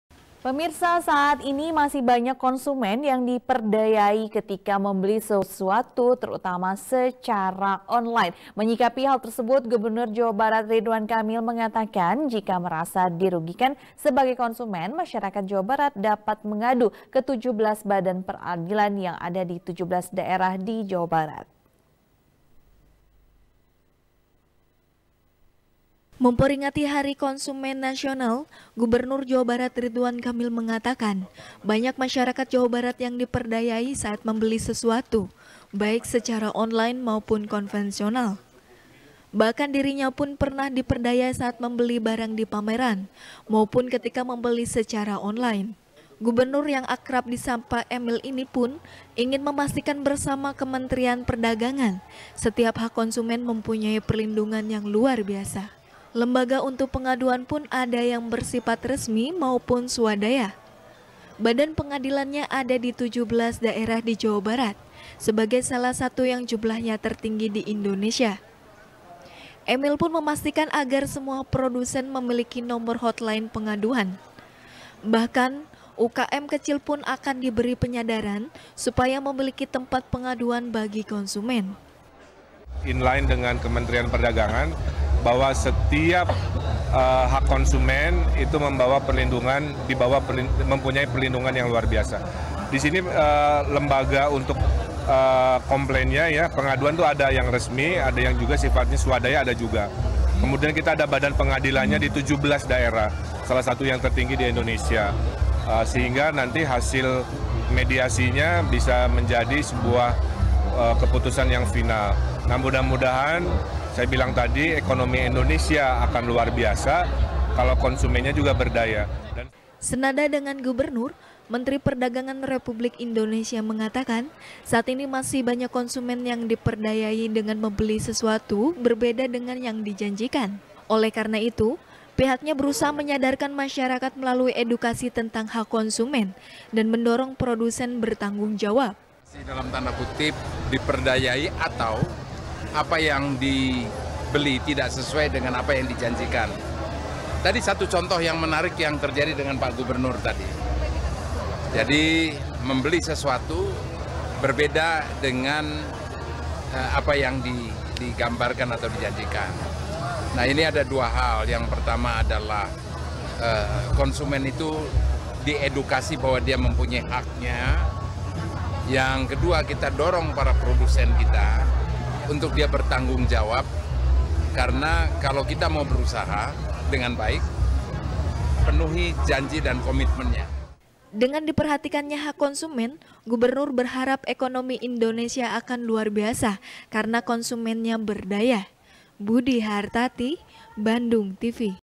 Dirugikan, Konsumen Dapat Mengadu Ke 17 Badan Peradilan | SBR | BANDUNGTV
SBR | BANDUNGTV | KRISANTI |saat ini, masih banyak konsumen yang diperdayai ketika membeli sesuatu terutama secara online. menyikapi hal tersebut, gubernur jawa barat ridwan kamil mengatakan, jika merasa dirugikan sebagai konsumen, masyarakat jawa barat dapat mengadu ke tujuh belas badan peradilan, yang ada di tujuh belas daerah di jabar.